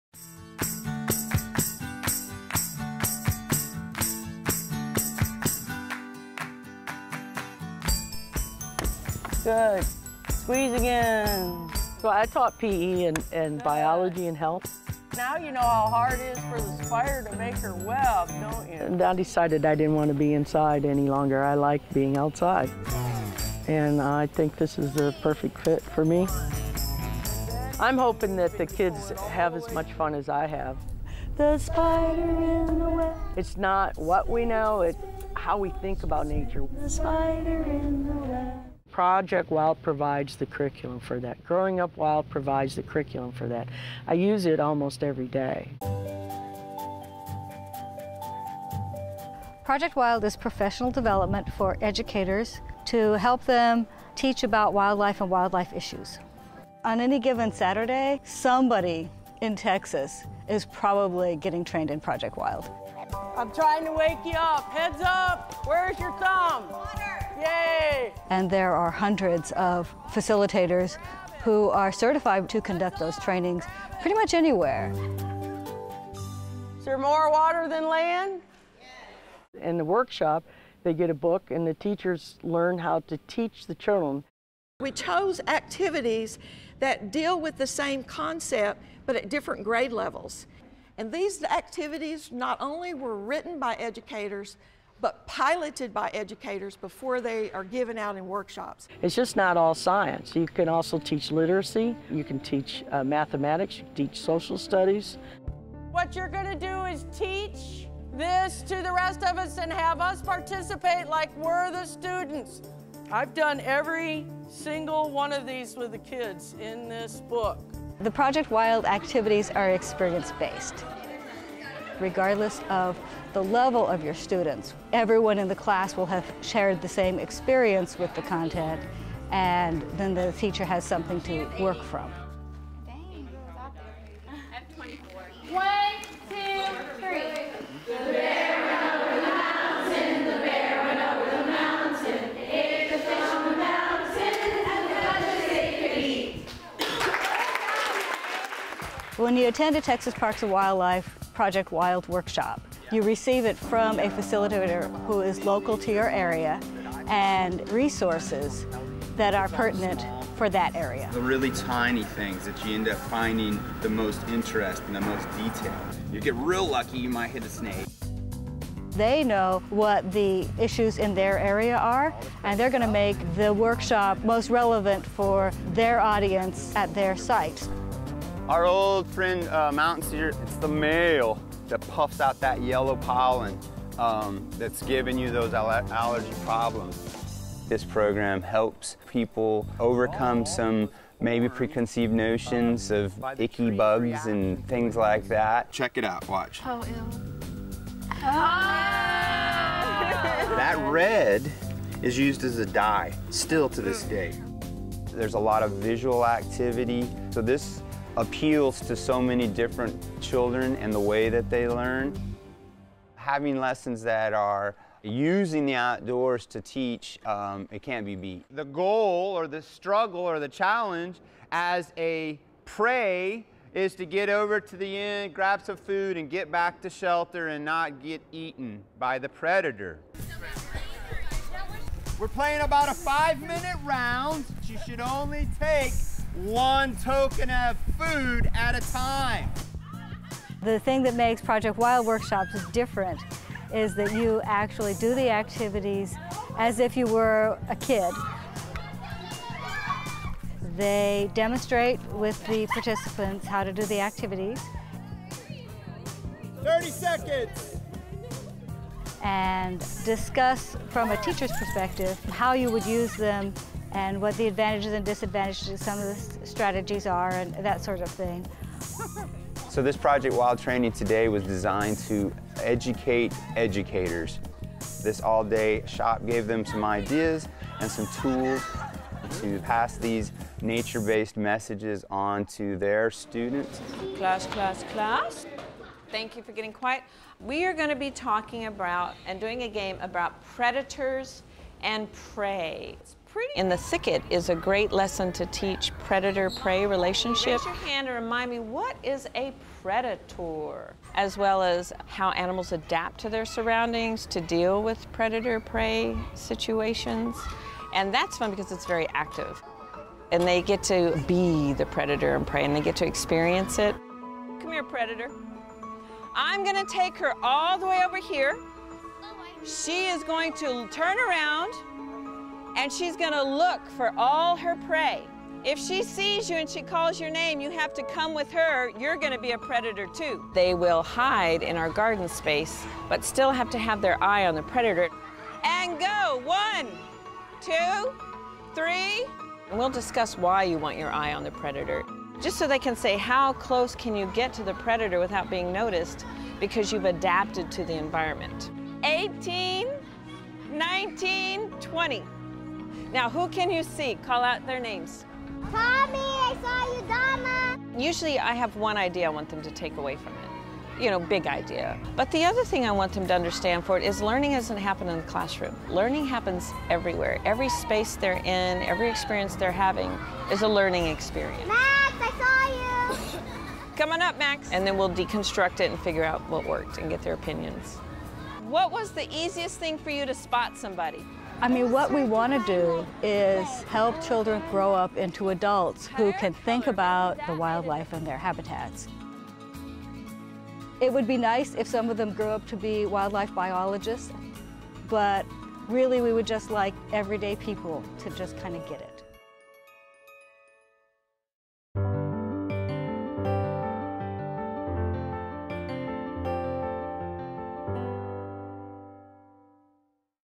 Educating the Educators, Project WILD
Educating the Educators
Children growing up in increasingly urban areas need to understand to role that wildlife plays in our world, but not all teachers know enough to teach it. ProjectWILD is empowering teachers with the lessons and activities they need to educate the next generation of wildlife stewards.